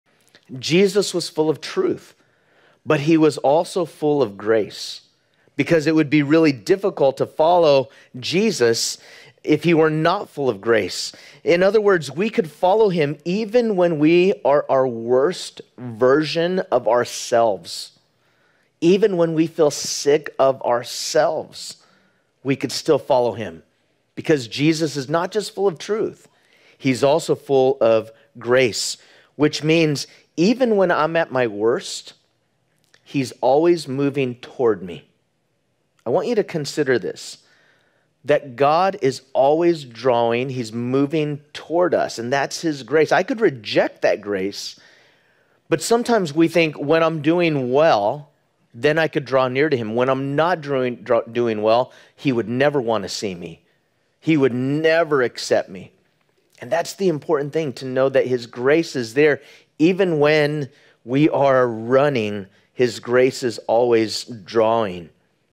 Truth and Grace
Jesus was full of truth, but He was also full of grace. We encourage you to rewatch the Christmas message as a reminder that we are able to draw near to God even when we feel like we can't.